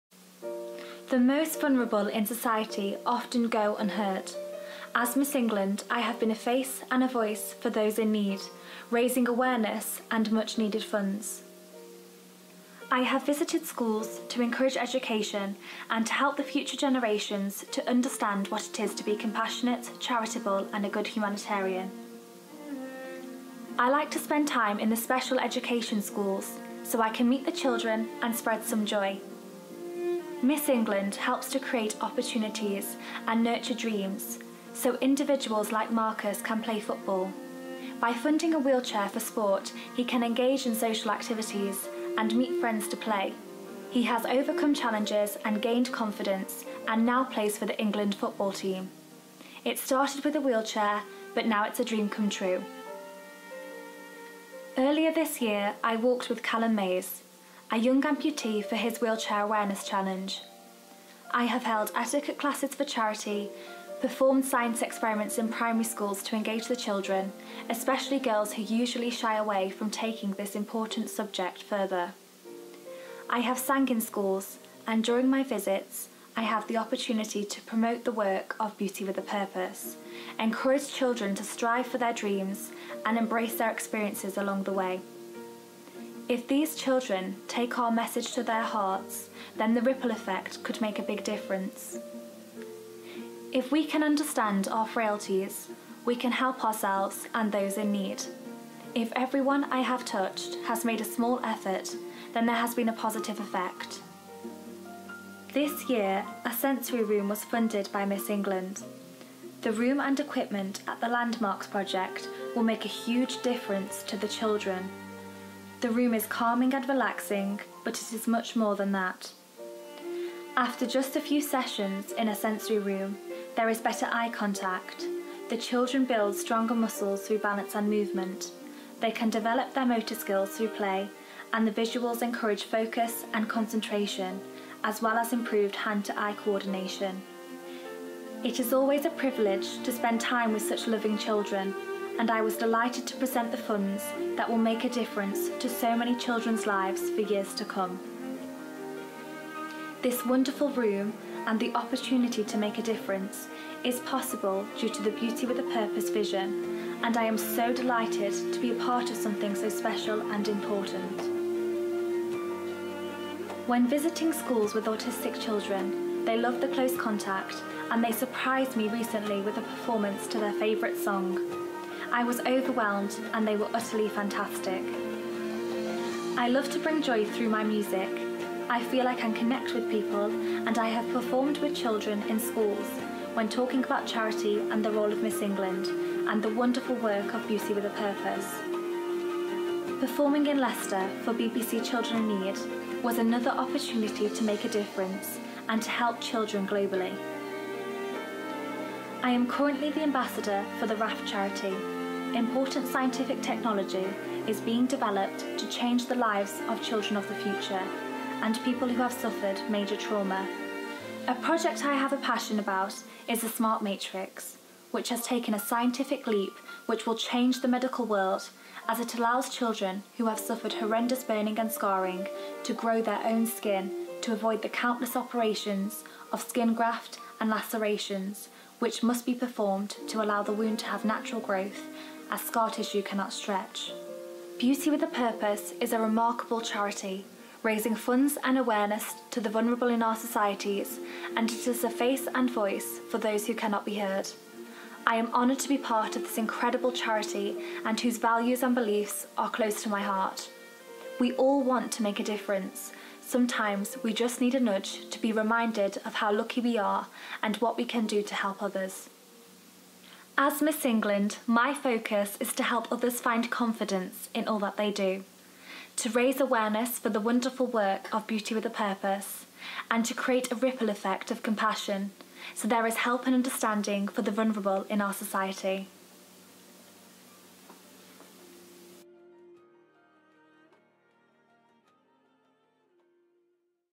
MW2015 : ENGLAND, Beauty with a Purpose Presentation
As varied as the contestants themselves, the projects are inspiring to behold, and are their 'Beauty with a Purpose'. Although there are many great projects, only one can be chosen as the winner of the Beauty with a Purpose Award, an accolade presented at the Miss World Final, on Saturday the 19th of December.